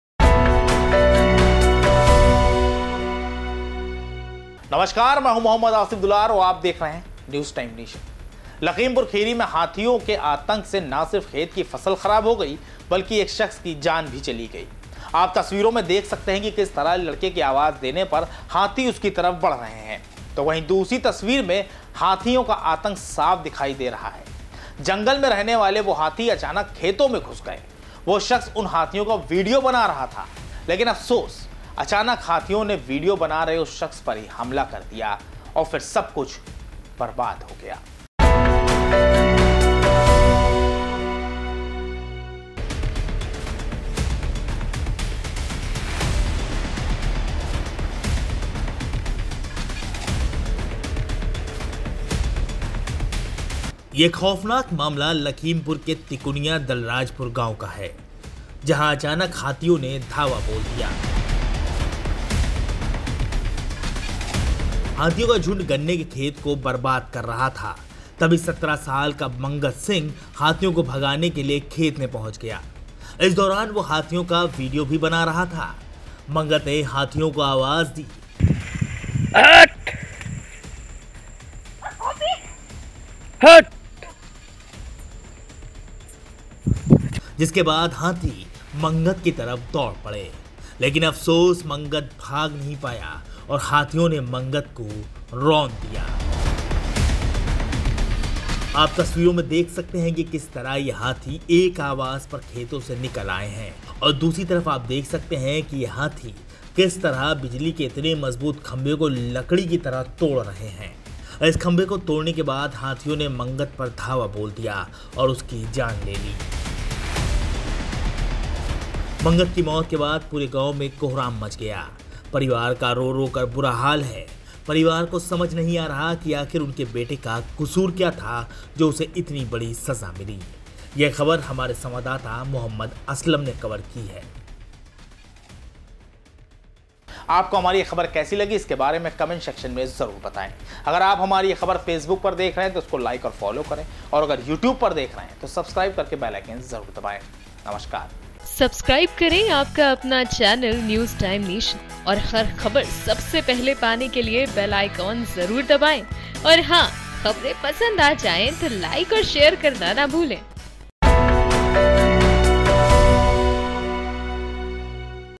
Lakhimpur : हाथियों के वीडियोने ली जान ! || News Time Nation
लखीमपुर खीरी में हाथियों ने के आतंक से ना सिर्फ खेत की फसल खराब होई गई बल्कि एक श्ख्स की जान भी चली गई...आप तस्वीरों मे देख सकते हैं कि किस तरह लड़के के आवाज देने पर हाथी उसकी तरफ बढ़ रहे हैं...तो वहीं दूसरी तस्वीर में हाथी आतंक साफ दिखाई दे रहा है... जंगल में रहने वाले वो हाथी अचानक खेतों में घुस गए...वो शख्स उन हाथियों का वीडियो बना रहा था...लेकिन अफसोस अचानक हाथियों ने वीडियो बना रहे उस शख्स पर ही हमला कर लिया...और फिर सबकुछ बर्बाद हो गया